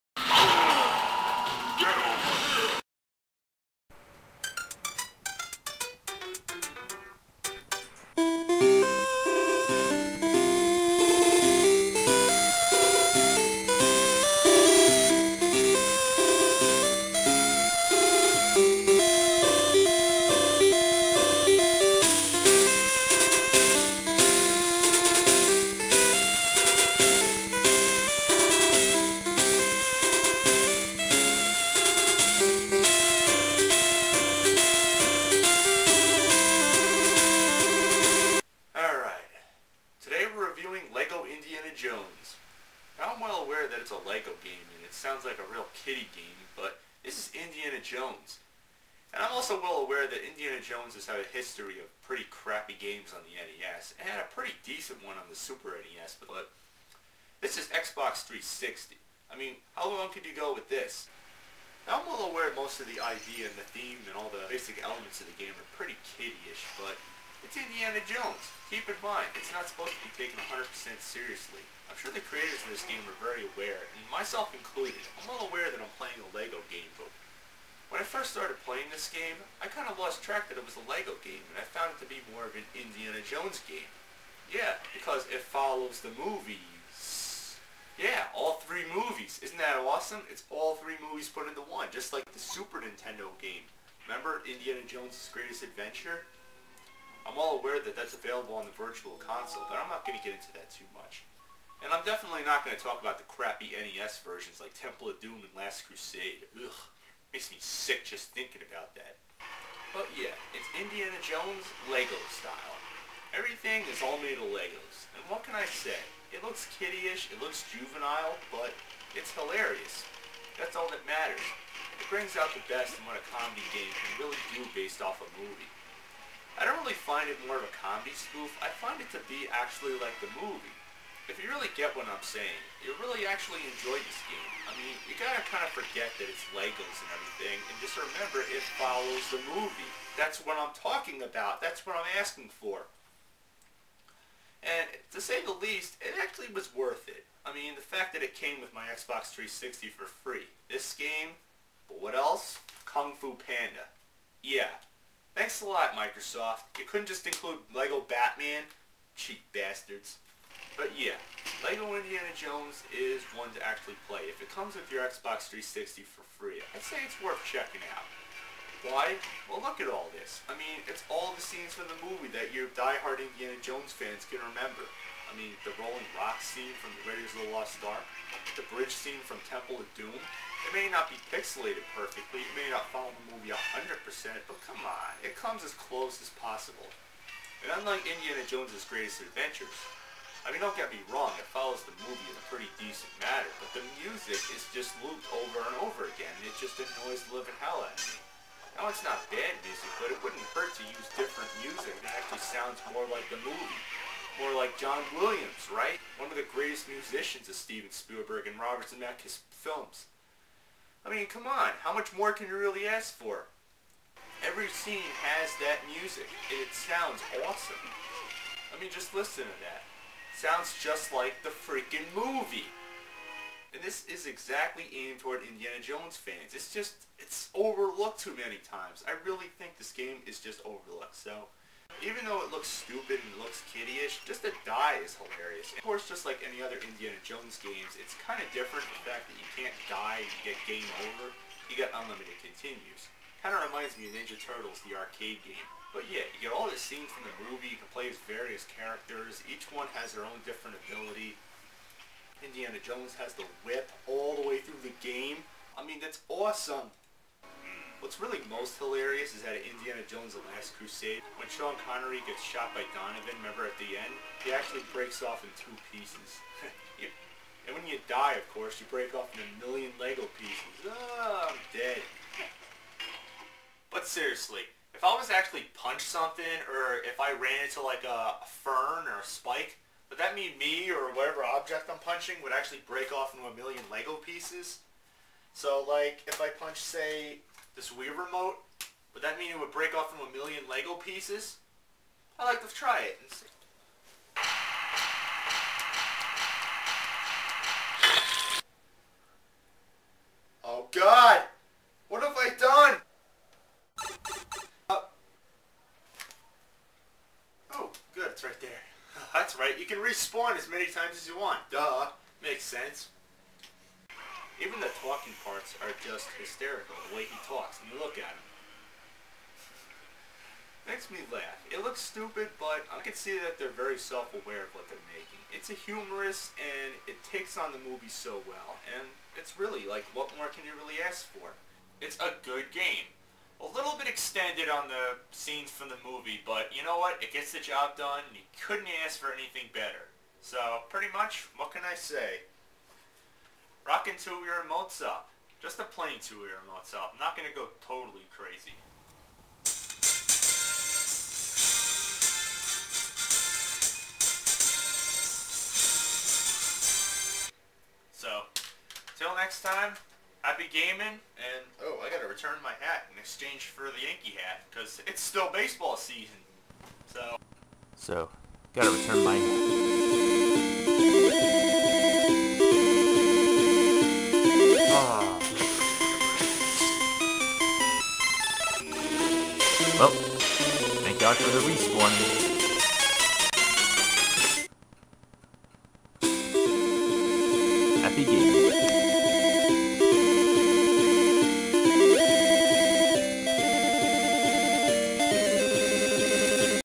Lego Indiana Jones Review
Wow! this one's an old one, but I figured since I did a review on Lego POTC I might as well reupload this one, just with a few changes here and there. Enjoy.